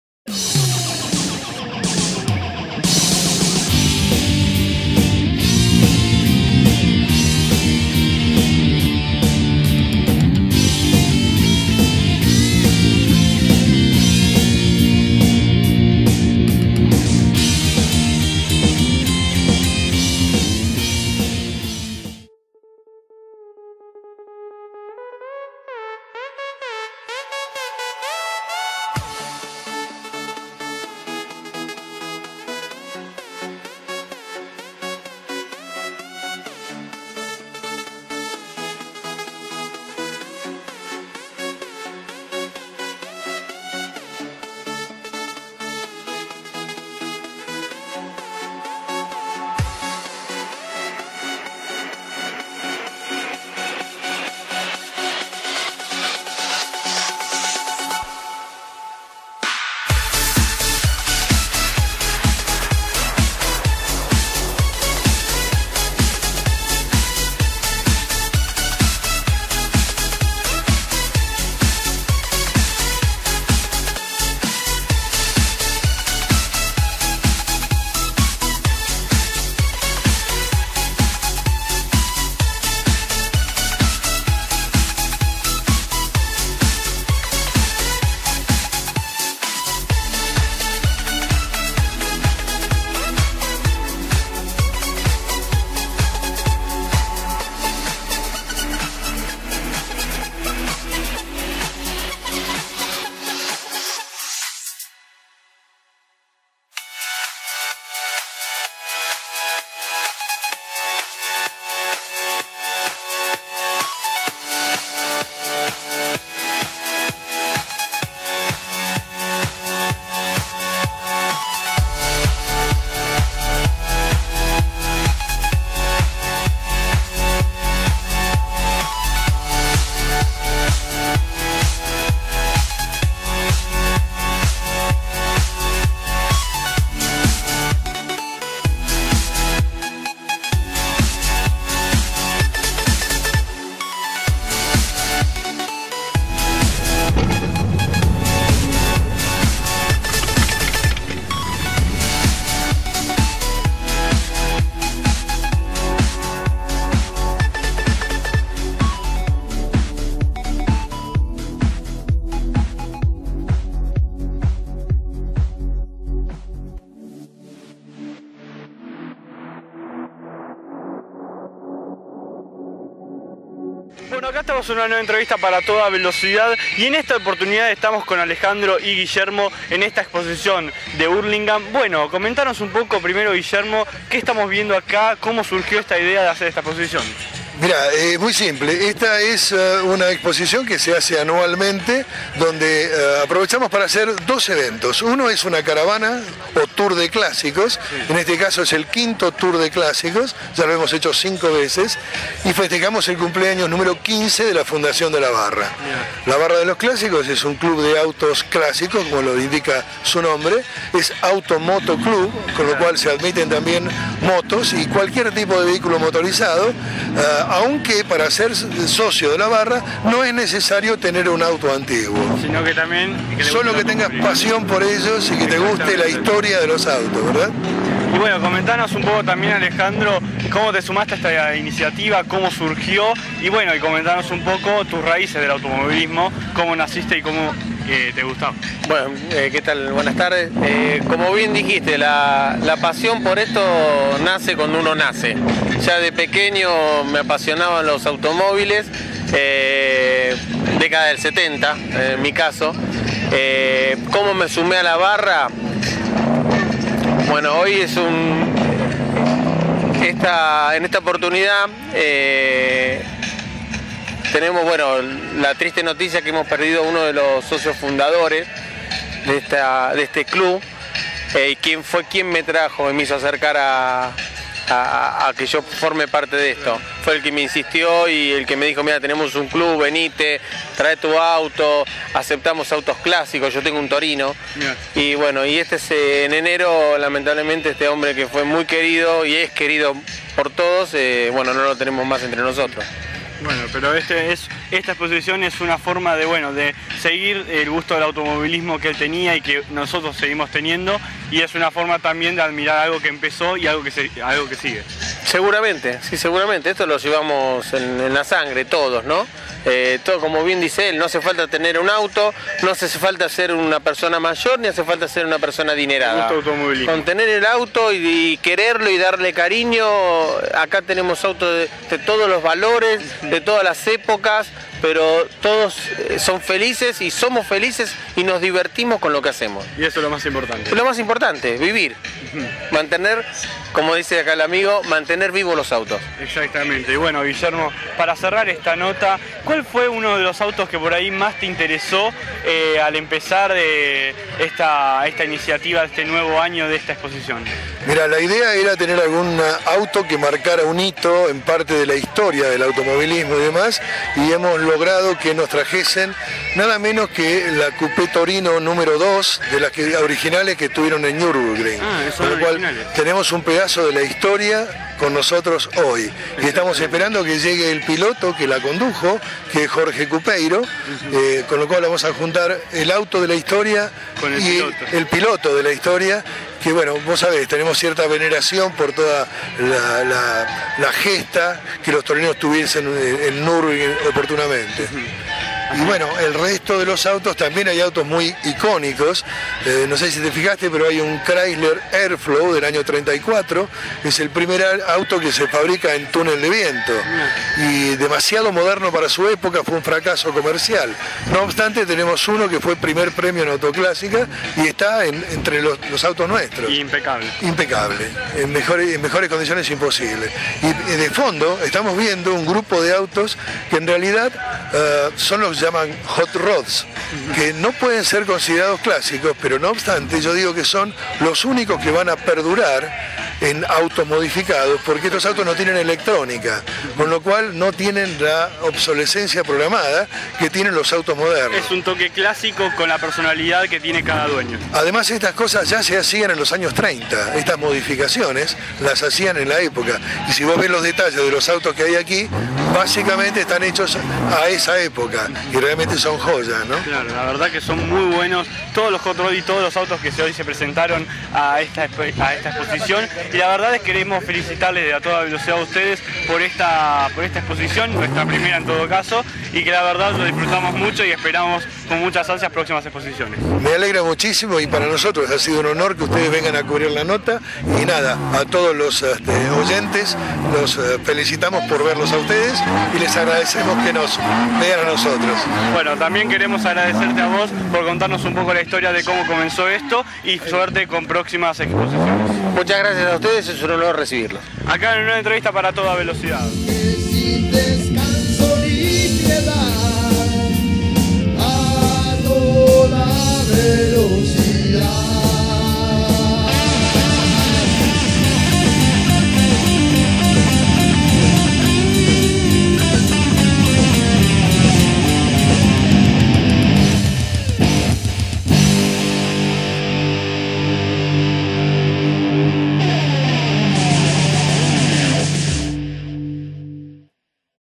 La Barra de los Clasicos - 5to. Tour de Clásicos por Hurlingham - Parte 1 - 04/2015.
En esta primer parte te mostramos la clásica caravana, nota con autoridades e imágenes de la exposición.